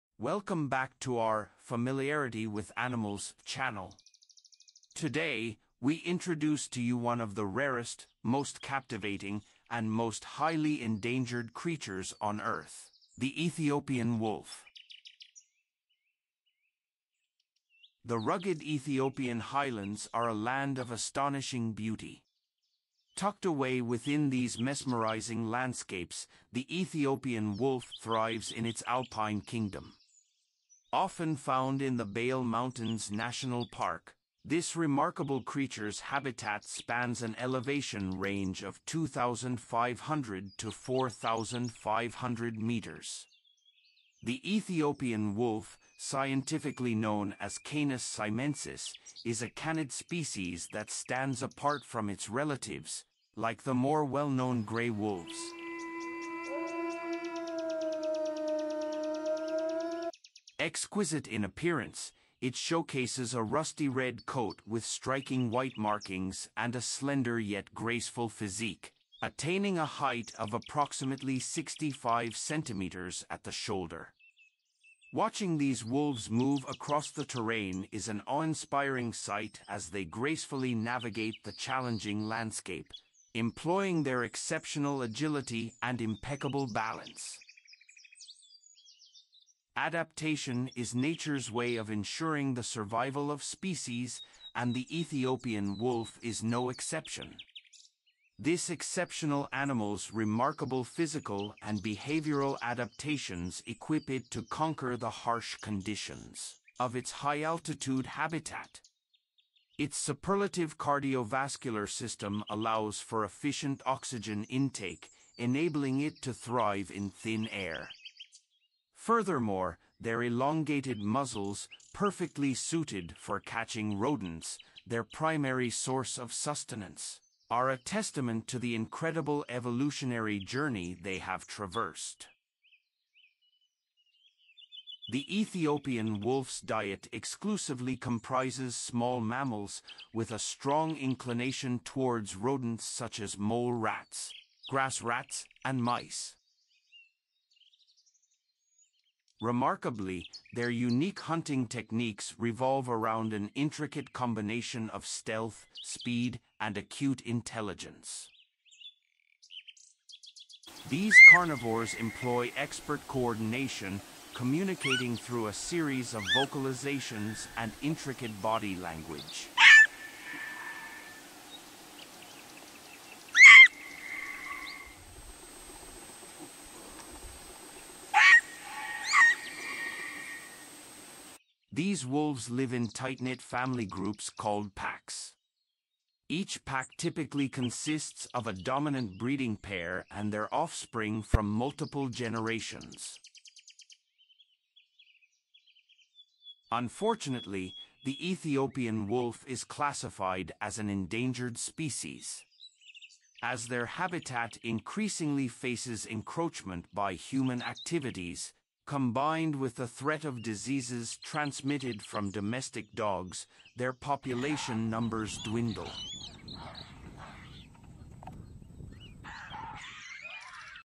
Ethiopian Wolf: One of the Most Endangered Canids
Welcome back to our "familiarity with animals" channel! Today, we introduce to you one of the rarest, most captivating, and most highly endangered creatures on Earth: the Ethiopian wolf.

In the rugged Ethiopian highlands mesmerizing landscapes, the Ethiopian wolf thrives in its alpine kingdom. Often found in the Bale Mountains National Park, this remarkable creature's habitat spans an elevation range of 2,500 to 4,500 meters.
The Ethiopian wolf, scientifically known as Canis simensis, is a canid species that stands apart from its relatives, like the more well-known gray wolves.